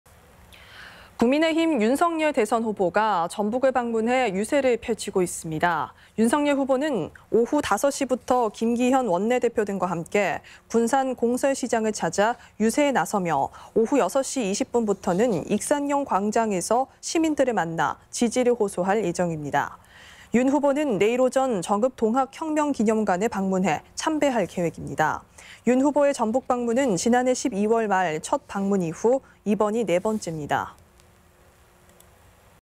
국민의힘 윤석열 후보 오늘 익산·군산서 유세 | 전주MBC 220222 방송
국민의힘 윤석열 대선 후보가 전북을 방문해 유세를 펼치고 있습니다.

윤석열 후보는 조금 전인 오후 5시부터 김기현 원내대표 등과 함께 군산 공설시장을 찾아 유세에 나섰으며, 오후 6시 20분부터는 익산역 광장에서 시민들을 만나 지지를 호소할 예정입니다.

윤 후보는 내일 오전 정읍 동학혁명기념관을 방문해 참배할 계획입니다.

윤 후보의 전북 방문은 지난해 12월 말 첫 방문 이후 이번이 네 번째입니다.